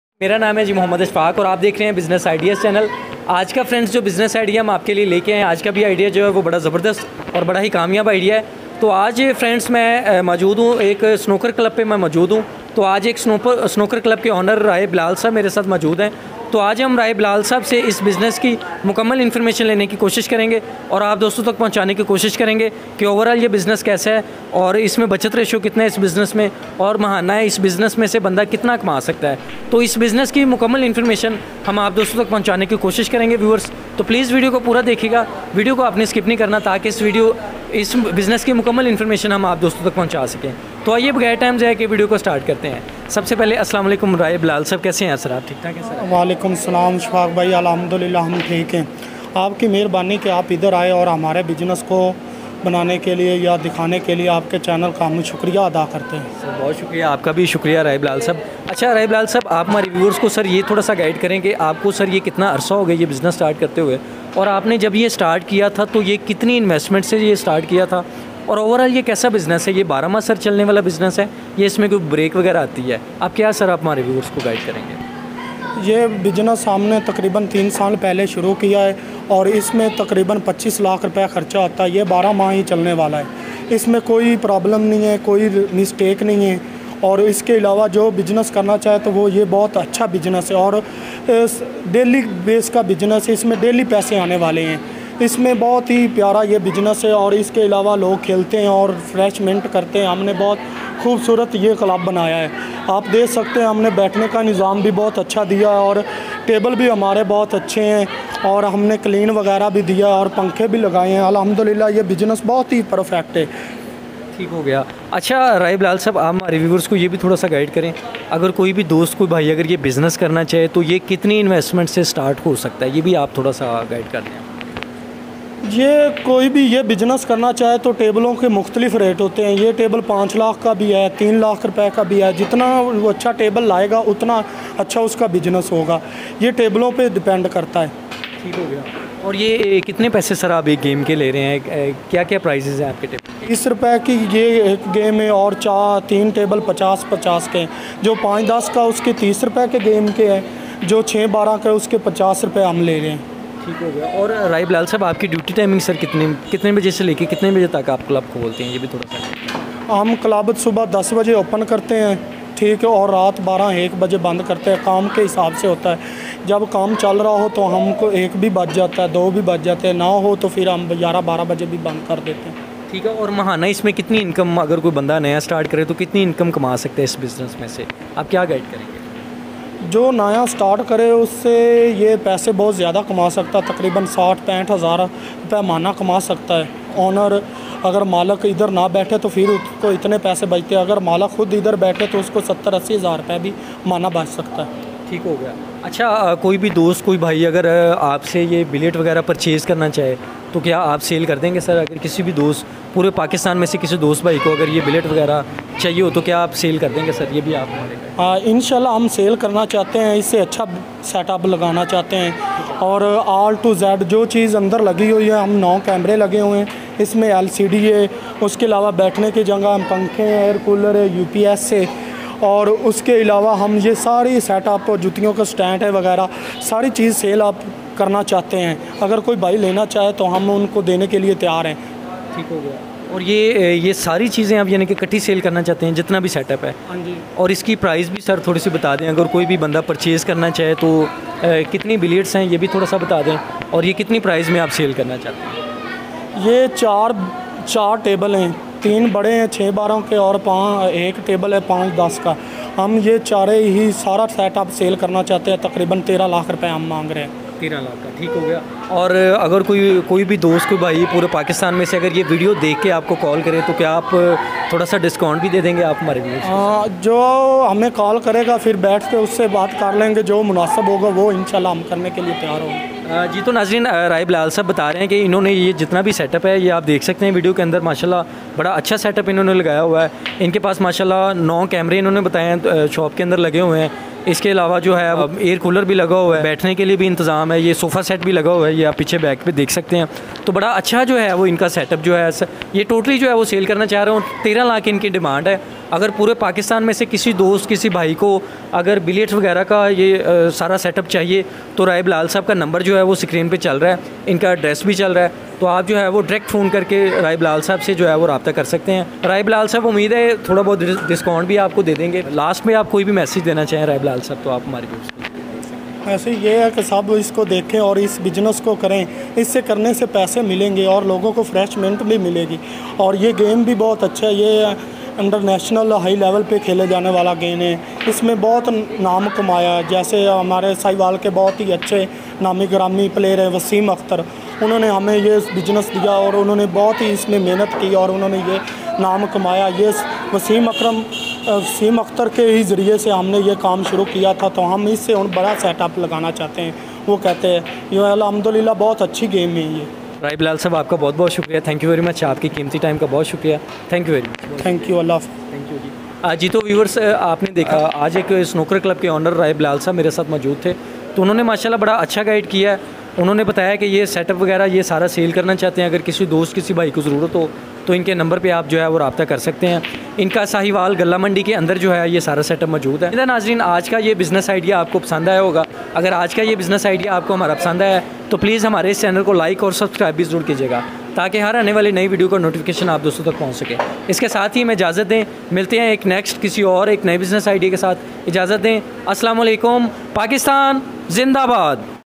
How To Start Snooker Club Business In Pakistan |Business Ideas
aj ki is video mein hum ap ko batayy gy bi or dikhayy gy bi ky snooker club ka business kaisa business hy or is business mein kitna mahana insan kama sakta hy to aj ki is video mein hum ap ko snooker club ki total information deny ki koshish kary gy freinds to plz ye video poori dekhiyy ga ta ky snooker club business ki total information ap tak pohanch saky thanks ap ka bhai muhammad Ashfaq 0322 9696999